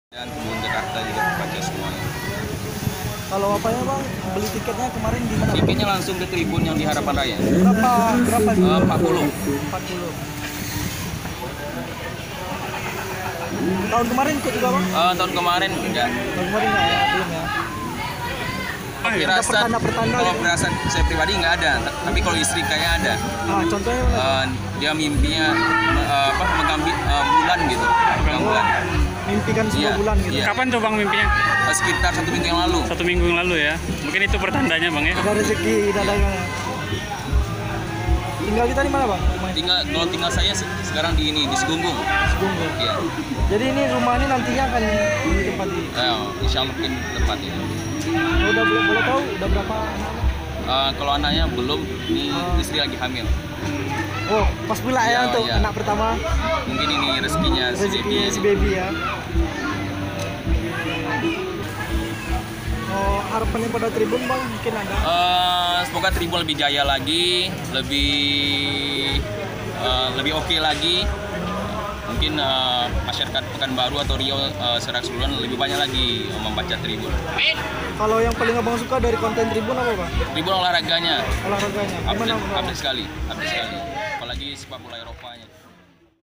Ini Peraih Hadiah Rumah Jalan Sehat HUT Tribun Pekanbaru
Beruntung nian nasib Suryadi, warga Kecamata Payung Sekaki Kota Pekanbaru ini mendapatkan satu unit rumah, Minggu (24/4/2017).